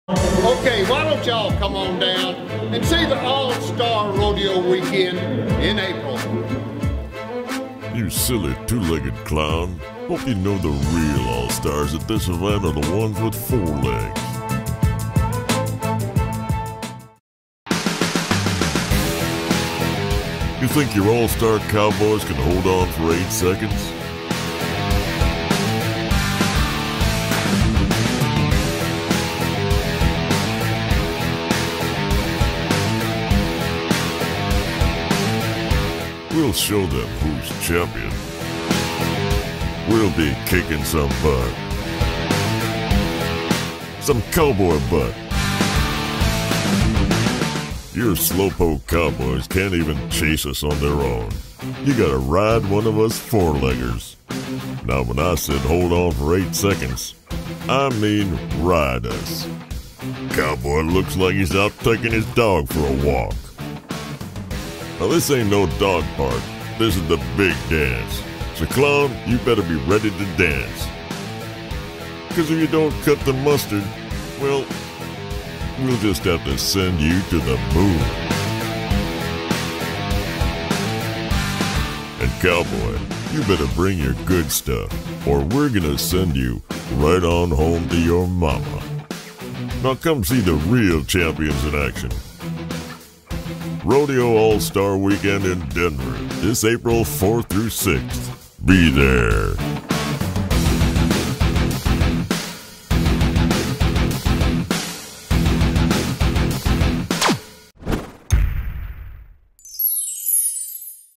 Rodeo All Star Bull Talk
Let the Bull speak! It's Champion vs Champion. We'll show you who the real All Star is. Rodeo All Star Weekend Denver April 4-6. Get your tickets today!